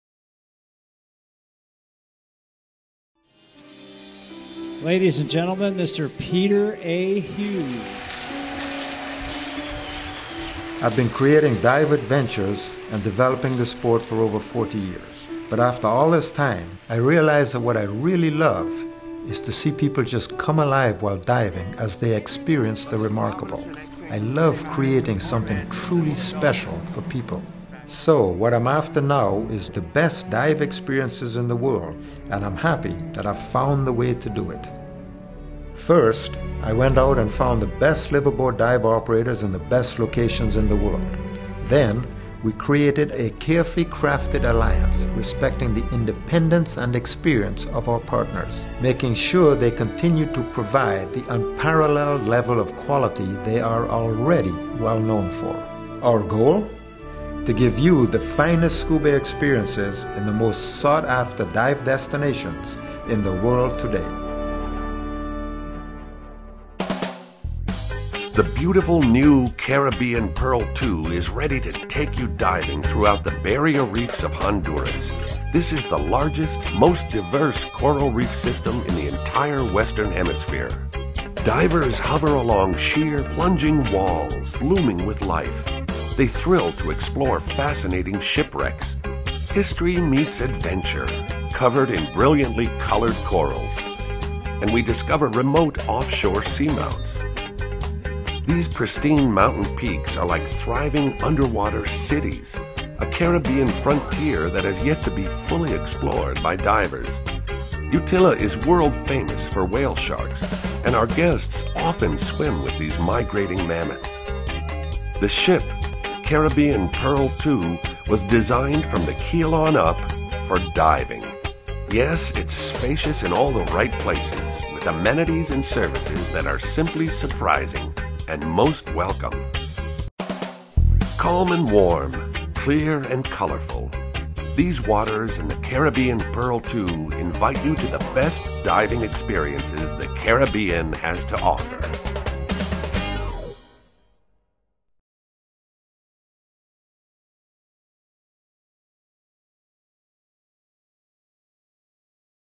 M/V Caribbean Pearl II
Proud Member of the DivEncounters Alliance, the M/V Caribbean Pearl II is based out of Utila, Honduras, the smallest of the Bay Islands of Honduras. The M/V Caribbean Pearl II cruises the Bay Islands delivering the best reefs, wrecks, and sea creatures to you. Accommodations include 1 Master Bedroom and 8 Deluxe Rooms. The Master Bedroom has a queen bed, individual climate control, private head and shower, and a hair dryer. All the Deluxe Rooms have a double bed, a single bunk, individual climate control, private head and shower and a hair dryer.